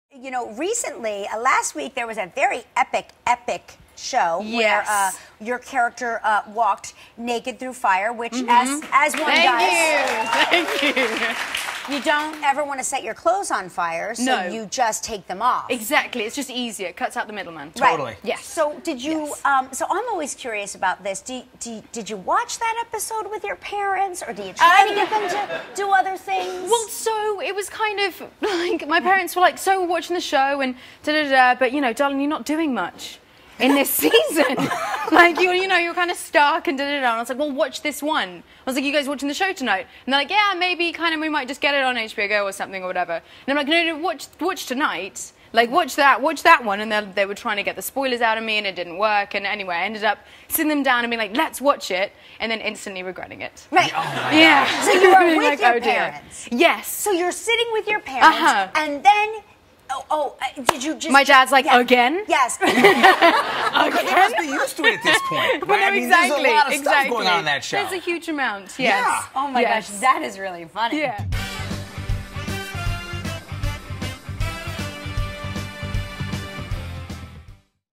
Emilia Clarke Regrets Watching "Game of Thrones" with Parents
Emilia Clarke talks about watching a nude "Game of Throne" scene with her parents.